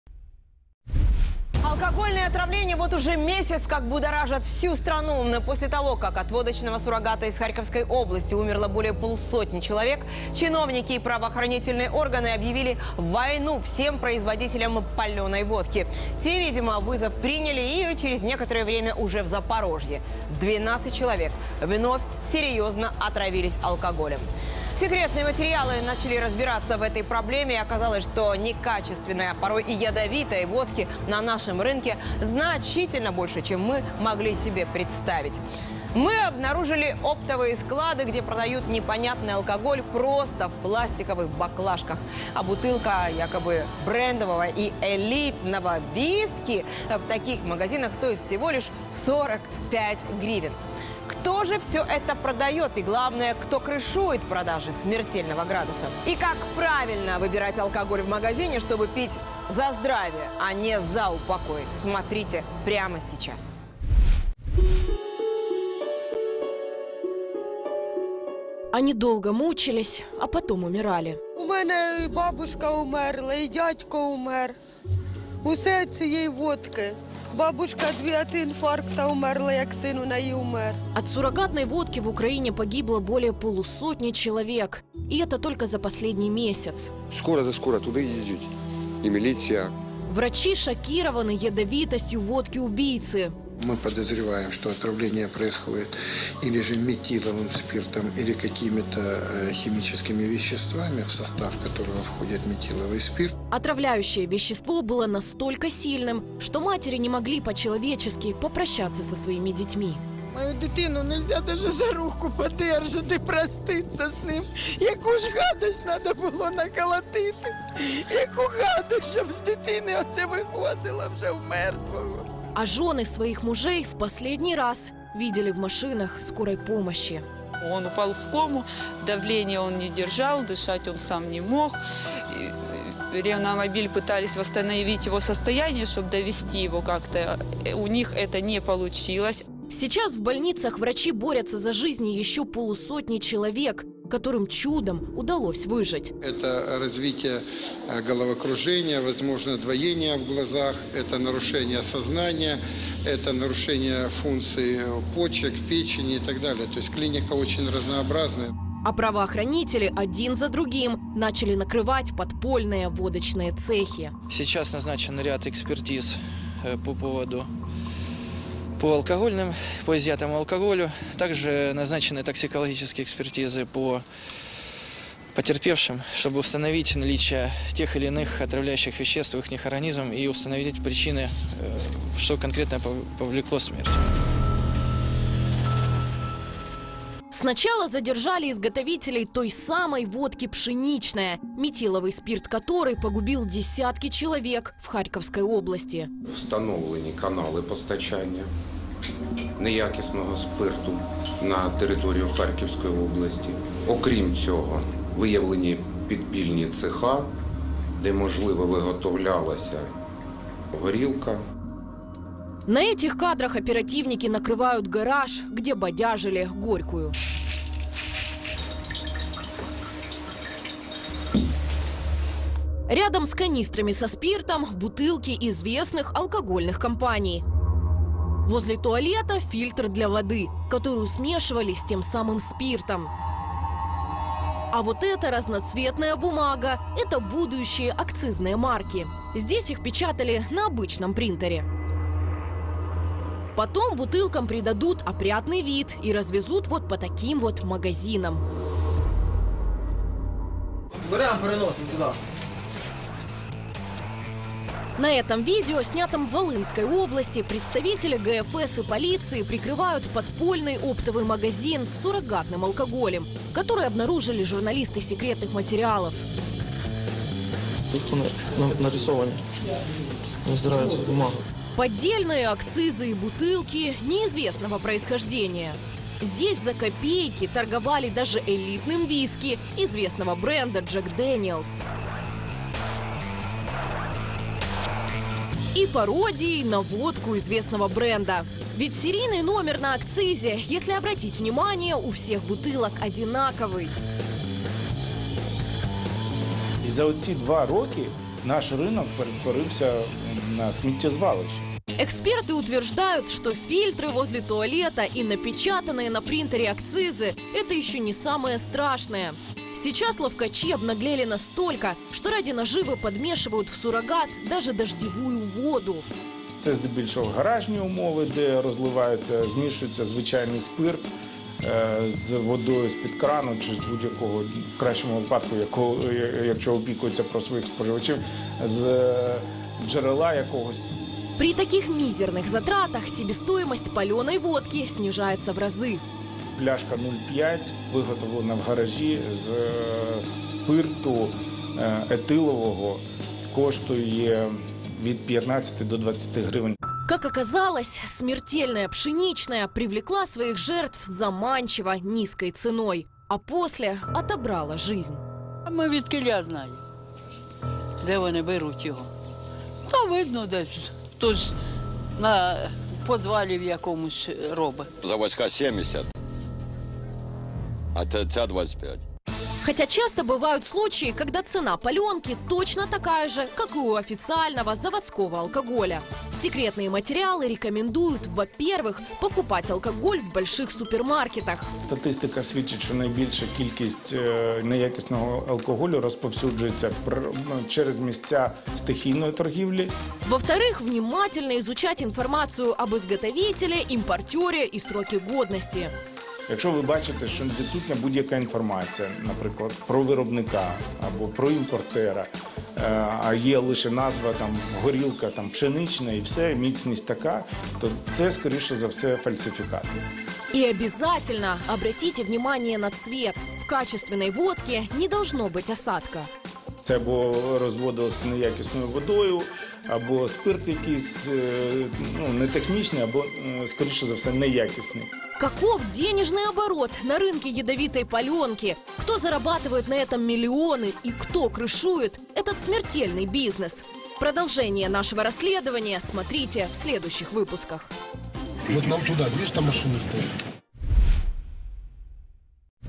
Хто продає і кришує продаж отруєного алкоголю
Алкогольні отруєння вже місяць розбурхують всю країну. Після того, як від горілчаного сурогату з Харківської області померло понад 50 осіб, чиновники та правоохоронні органи оголосили війну паленому алкоголю. Секретні матеріали почали розбиратися в цій проблемі, і виявилося, що неякісної горілки на вітчизняному ринку набагато більше, ніж можна було уявити. Подробиці - в сюжеті.